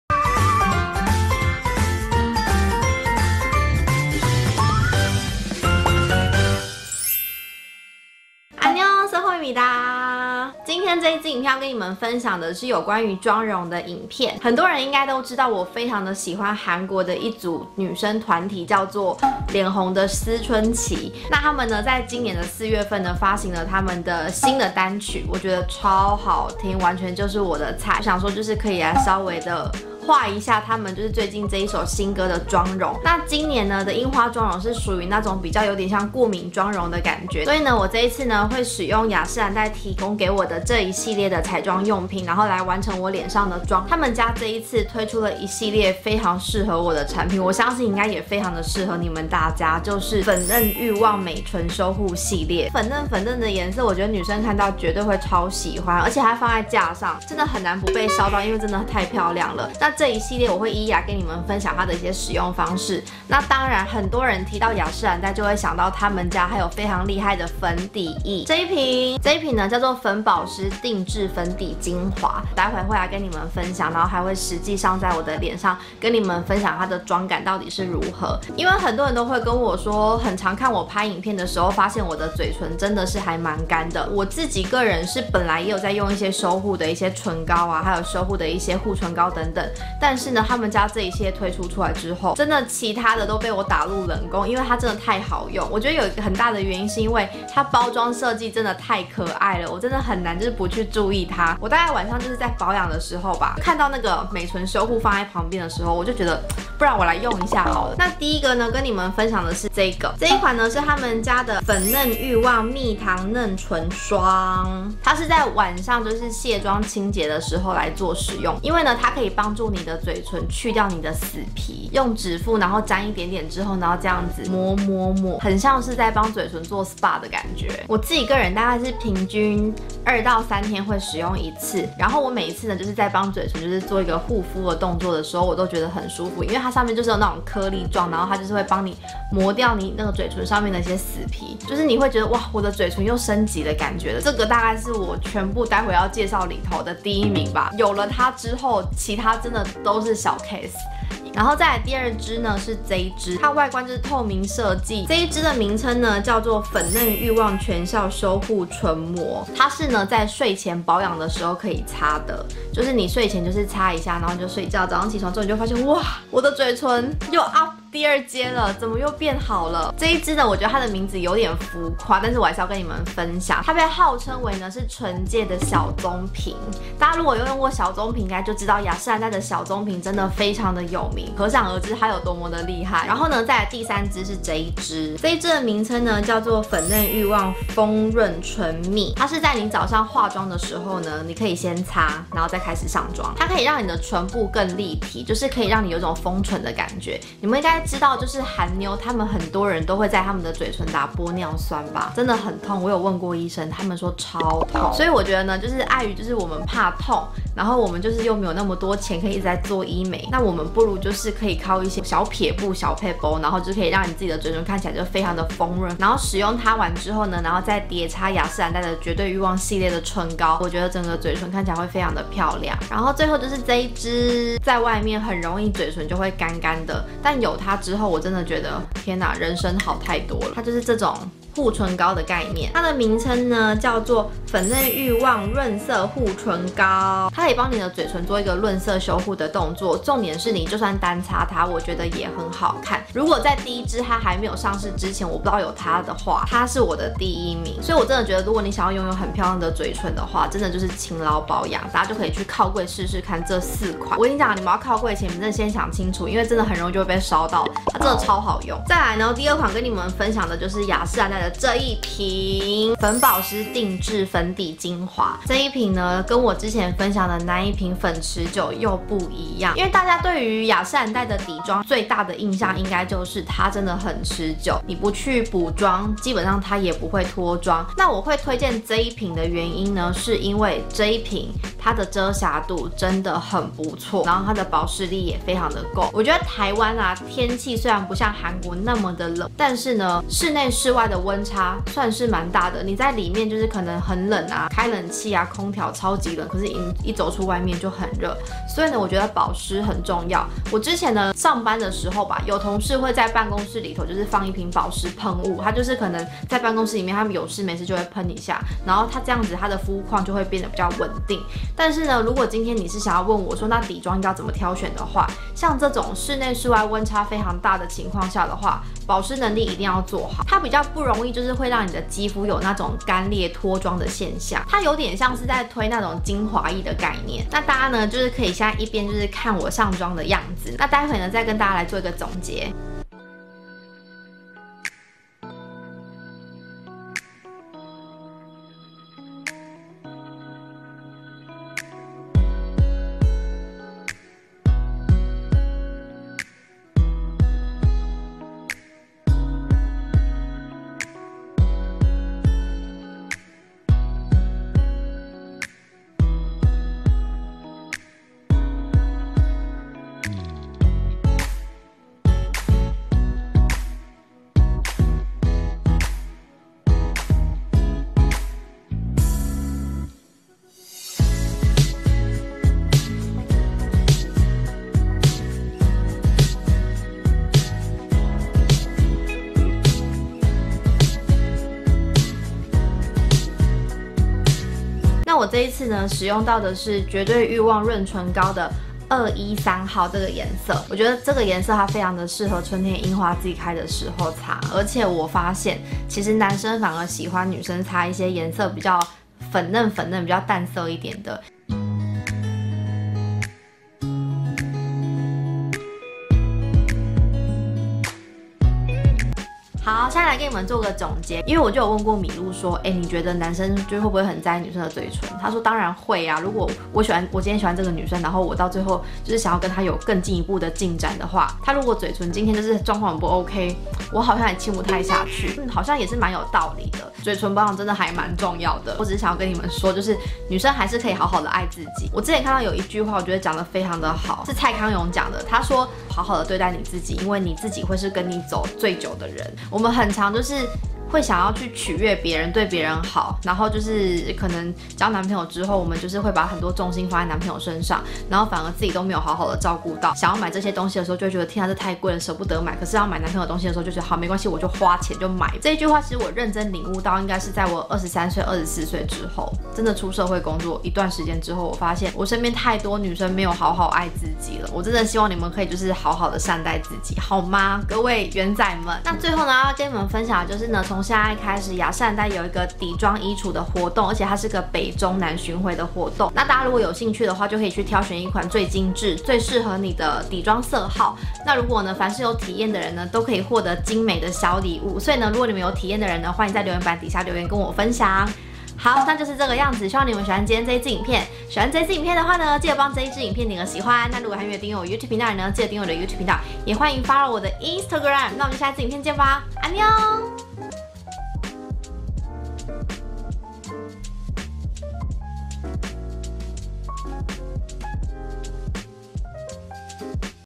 我的乾嘴唇終於有救了？性感嘟嘟唇要靠它！+畫一個韓妞的櫻花過敏妝 feat. 雅詩蘭黛
安妞！

這支影片拍完隔天我立刻剪出來
因為實在太想分享了!
不知道你們跟我是不是一樣都有乾嘴唇困擾
我自己真的用了很多方法都覺得沒這系列好用！

雅詩蘭黛新上市的粉嫩慾望美唇修護系列
24HR隨著不同時間搭配使用
配上這次也一起跟你們分享的粉保濕訂製粉底精華
perfect!!

剛好藉由這次我的最愛臉紅的思春期發新歌
一起來個韓妞的櫻花過敏妝容吧!!